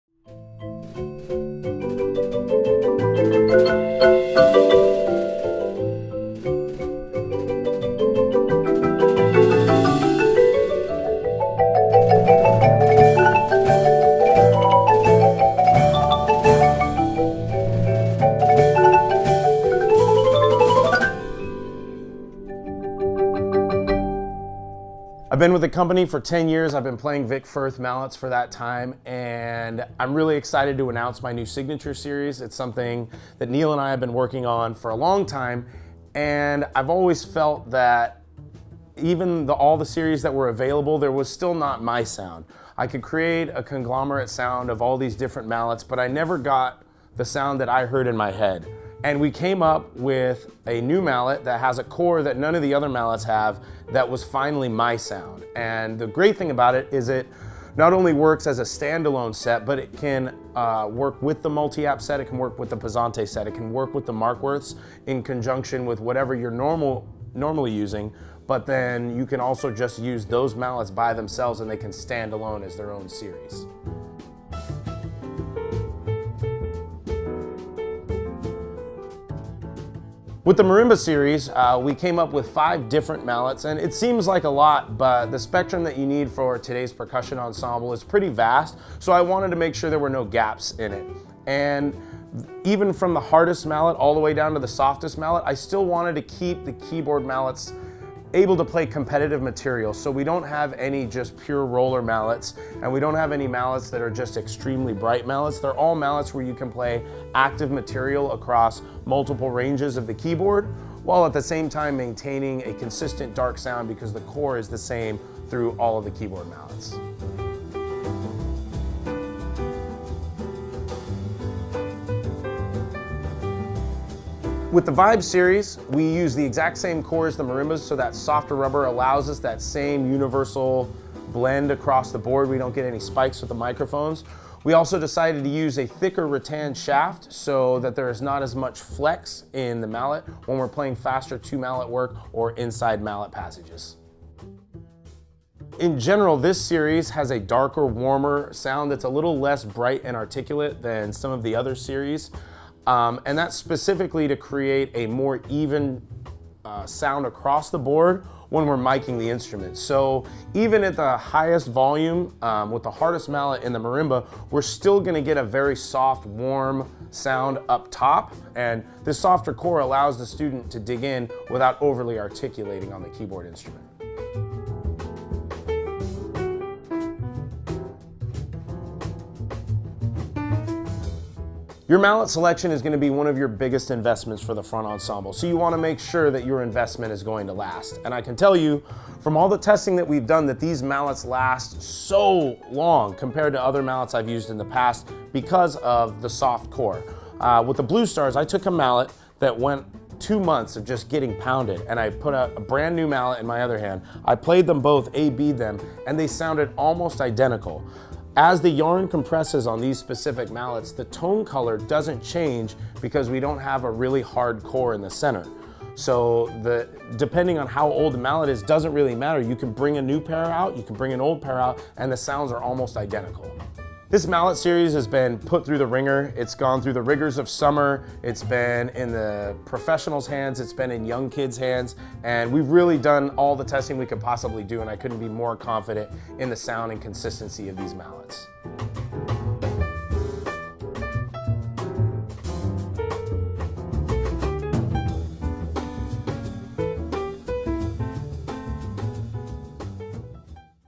Product Spotlight: Ian Grom Corpsmaster® Keyboard Mallets
Vic Firth is pleased to announce the NEW Ian Grom Corpsmaster® Signature series of mallets!

Ian Grom is a composer and educator whose artistic contributions extend into all seasons of the marching activity! Together with his work in the WGI arena – leading Chino Hills High School and Pulse Percussion to 5 World Championships – Ian’s efforts composing award-winning marching band shows and arranging for some of the world’s best DCI corps have cemented his reputation as one of the brightest minds in marching music today.

“There are so many sound choices within Vic’s keyboard lineup,” states Grom. “Still, I felt there was room to expand the offering and create something totally new. Amplification plays a big role with my marching ensembles. I wanted to create a series that would generate a warm and dark sound, even at the loudest dynamic levels. And, we have done it – these mallets feel great and sound huge!”

The Ian Grom Signature Series features 8 models: 5 for marimba and 3 for vibraphone. All models share the same unique warm rubber core for a superior universal blend and even quality across the range of the instruments.